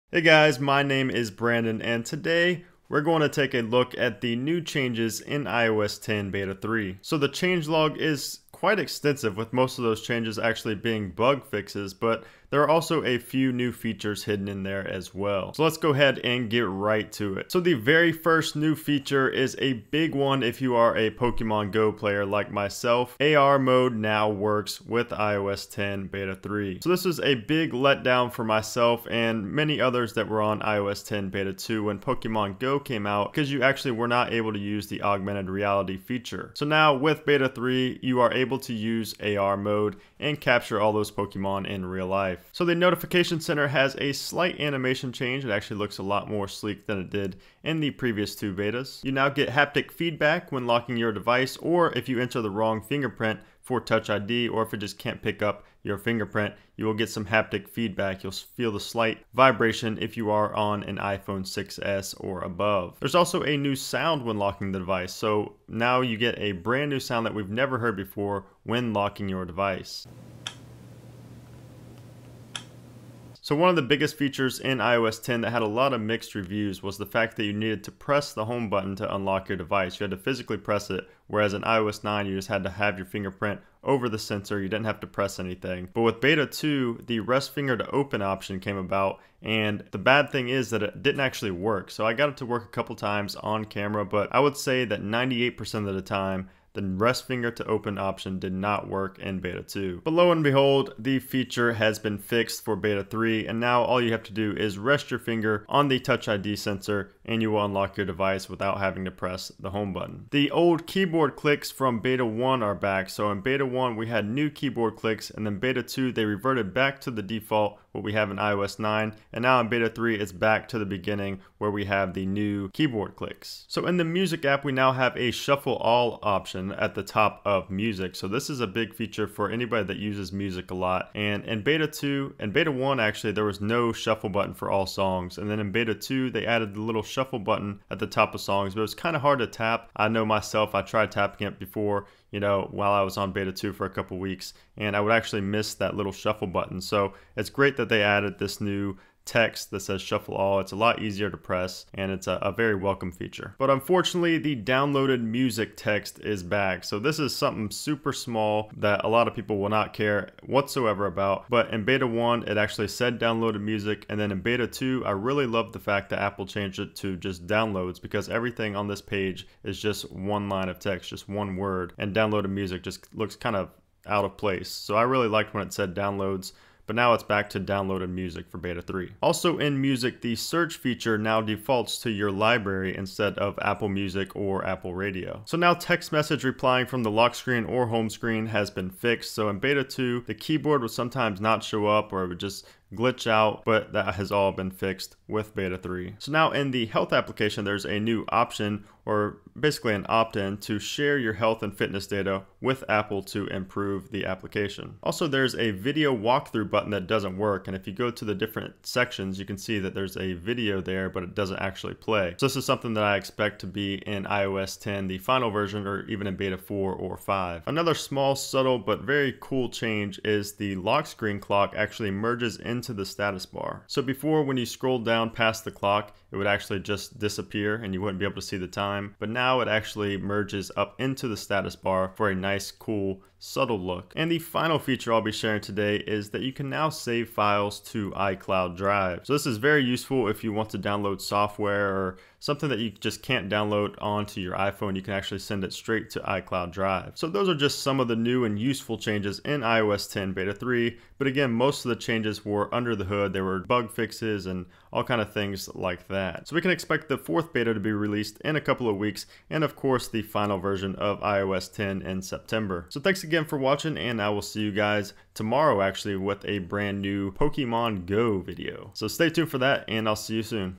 iOS 10 Beta 3 - What's New? (Changes/Features)
Here are the useful new features/changes in iOS 10 Beta 3!

Again, mostly bug fixes but there are also quite a few new features and changes added into iOS 10 beta 3.

STAY UP-TO-DATE!